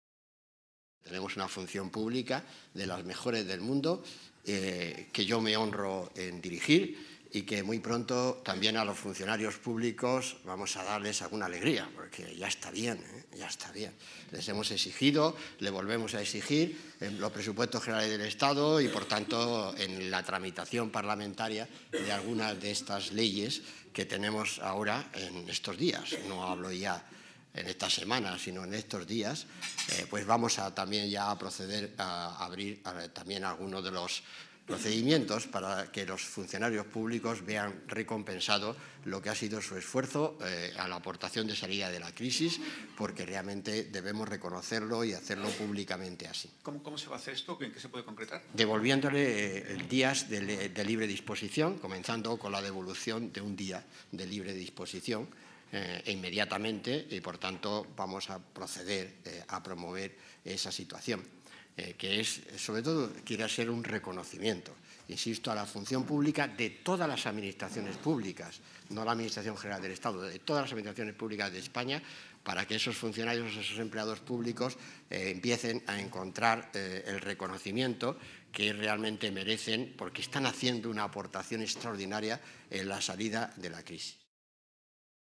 Montoro devolverá a los funcionarios un 'moscoso'
El ministro de Hacienda y Administraciones Públicas, Cristóbal Montoro, ha anunciado que el Gobierno procederá a devolver "inmediatamente" un día de libre disposición, los llamados 'moscosos', a través de alguna de las leyes que están ahora en tramitación parlamentaria.